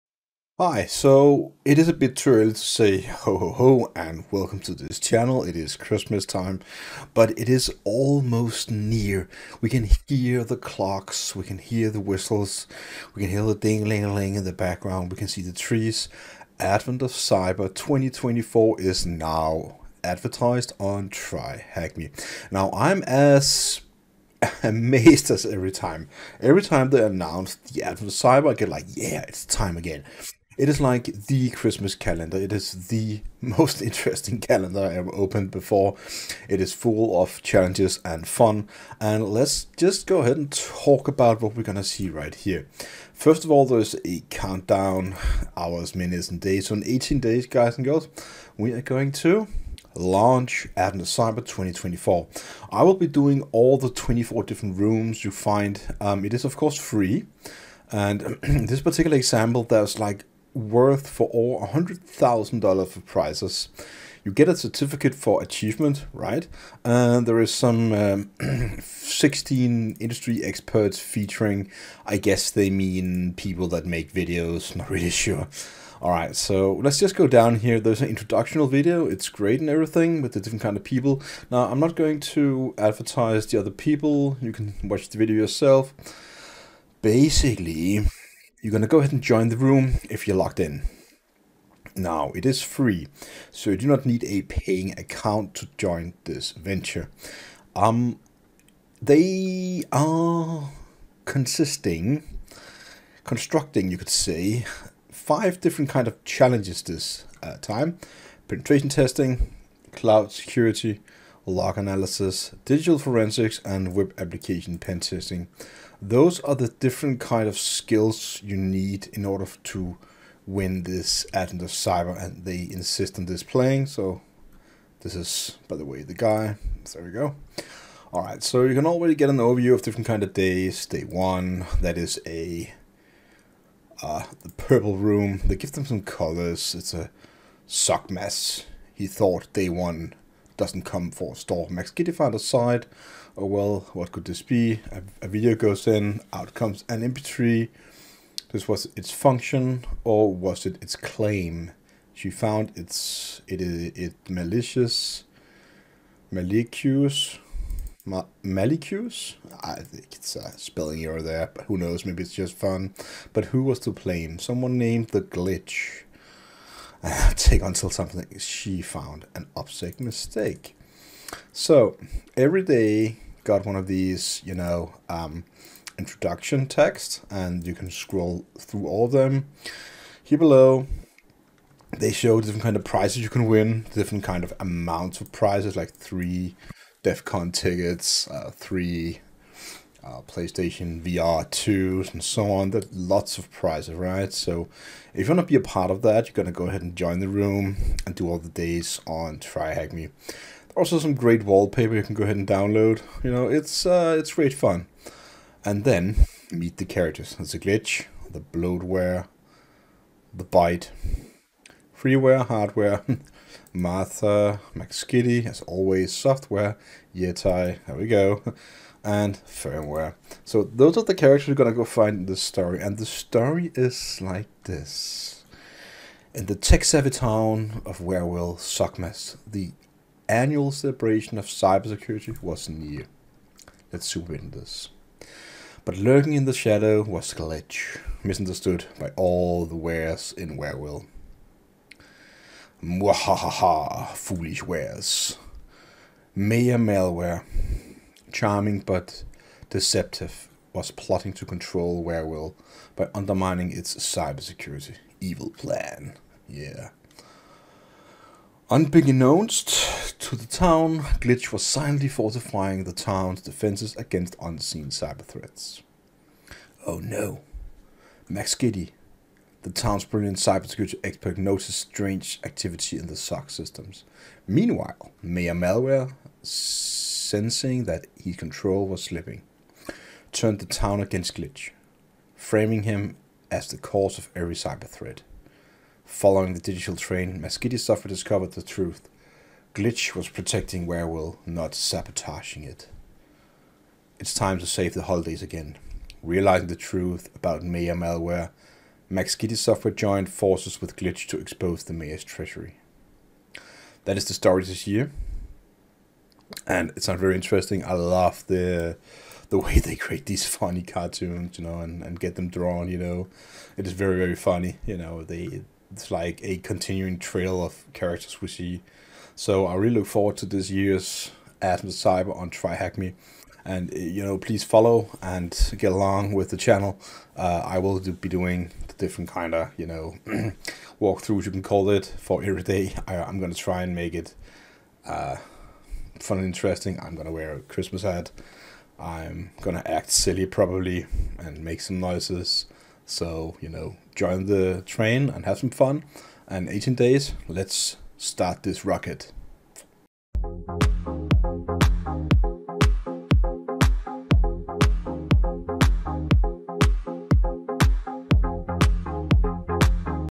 Introducing - Advent of Cyber 2024 - TryHackMe
Welcome to Advent of Cyber 2024 with TryHackMe! 🎄🔐 Get ready for a lot of days of cybersecurity challenges that help beginners and pros alike level up their hacking skills. In this series, we’ll dive into daily tasks, covering essential skills in areas like networking, cryptography, web vulnerabilities, and more. Each challenge builds your knowledge step-by-step, with solutions explained in a beginner-friendly way. Whether you're a cybersecurity enthusiast or someone wanting to break into the field, this is the perfect event to boost your skills!

Join me as we tackle the challenges, learn together, and unwrap new cybersecurity skills each day.